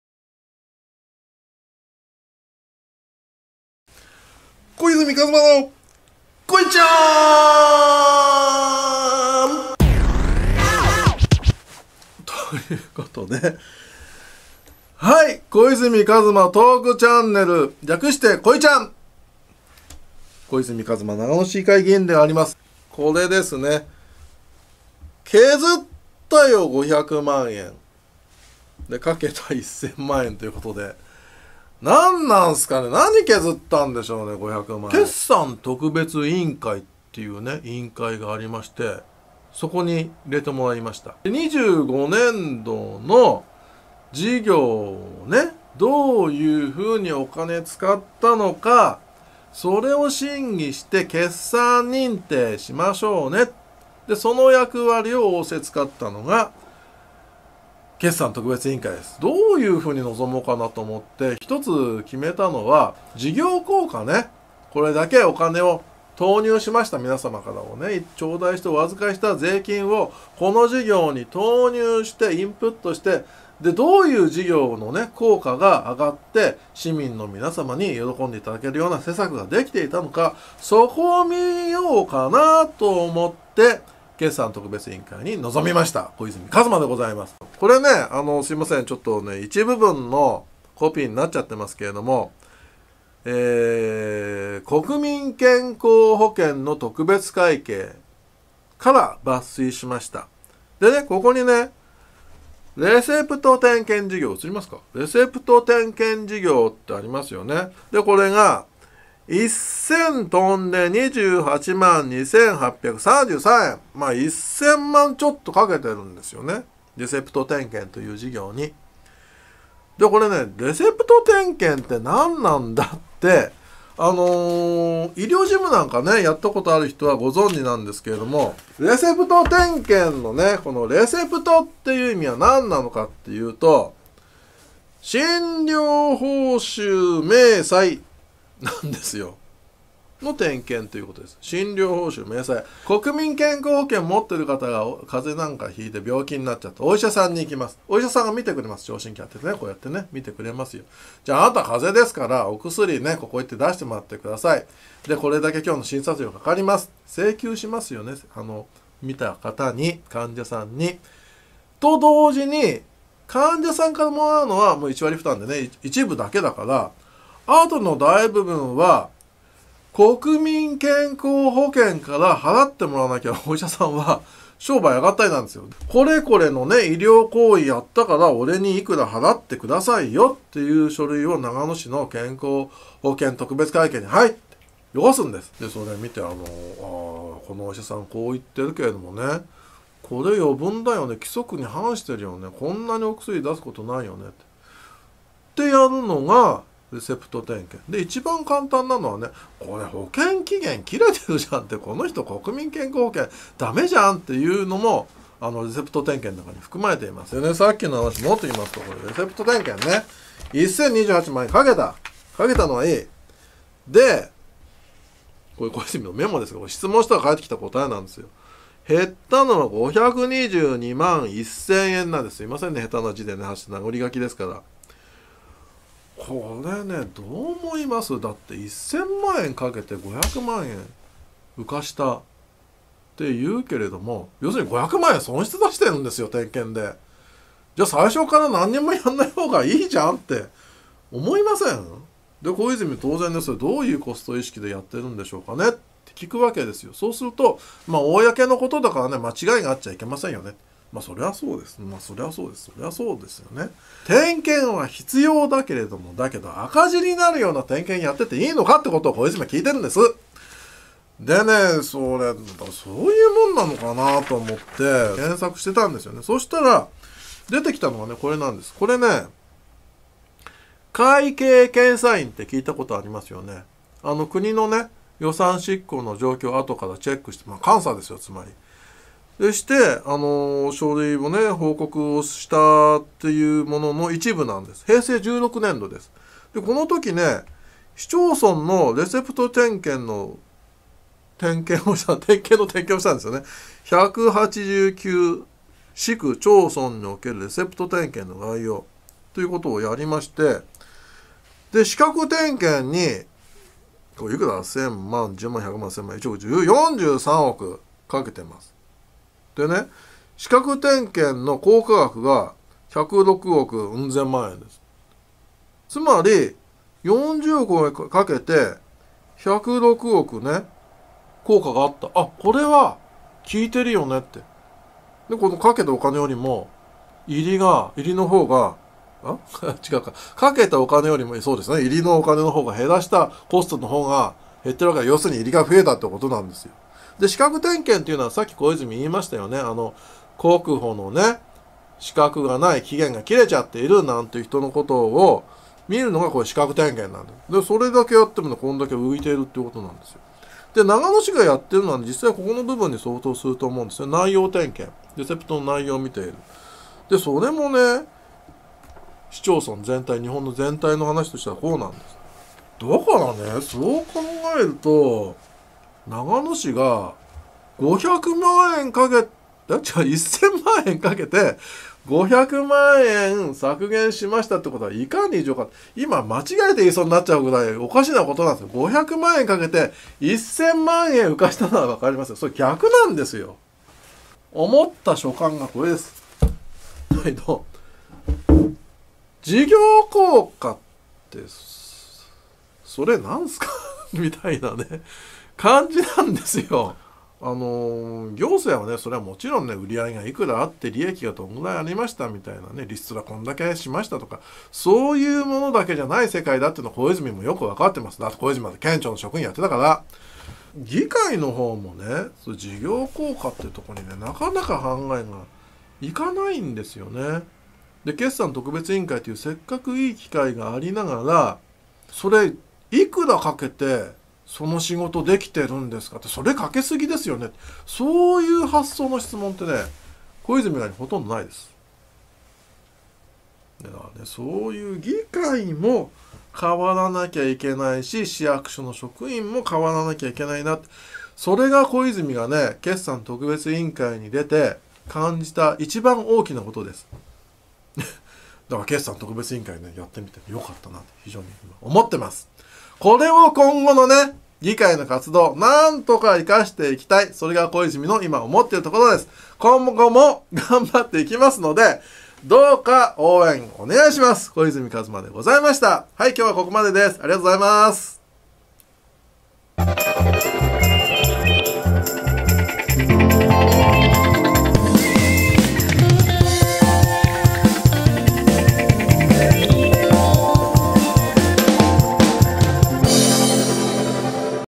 【こいちゃん #22】 削った500万円、かけた1000万円－国保会計にコスト意識なし
500万円コスト削減するために、人件費1000万円かけている長野市の国民健康保険­特別会計。レセプト点検のお話。【長野市議会議員・小泉一真とーくちゃんねる】